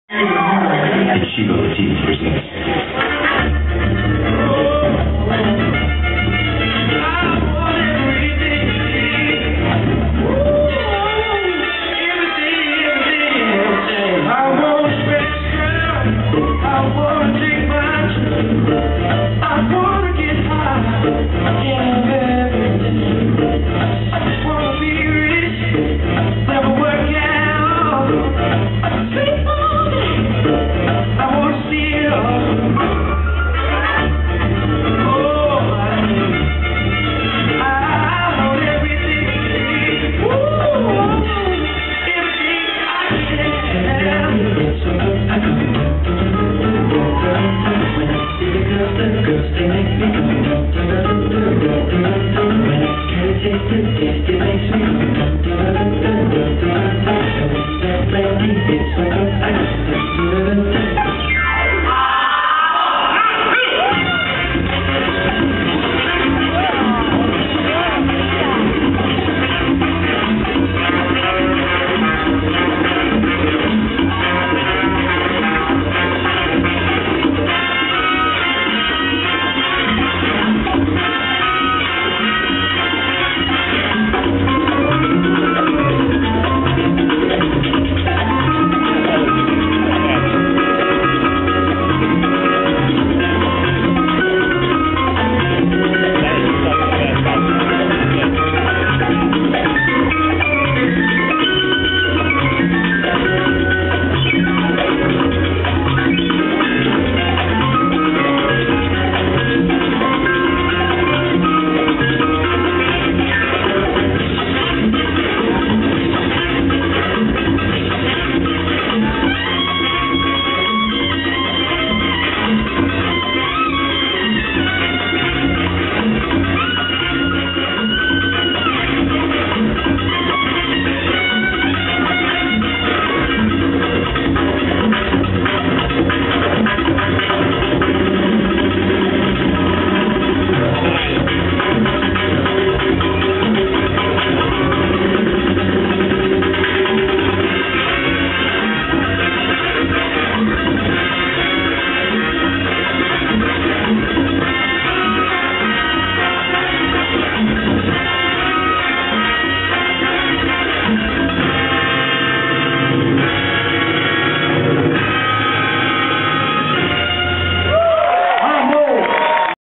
Nery and Giana Montoya of Shiva Latina from Sacramento California at the IHSC '10
My good friend Nery and his partner did their first routine ever! he is such an amazing dancer! enjoy!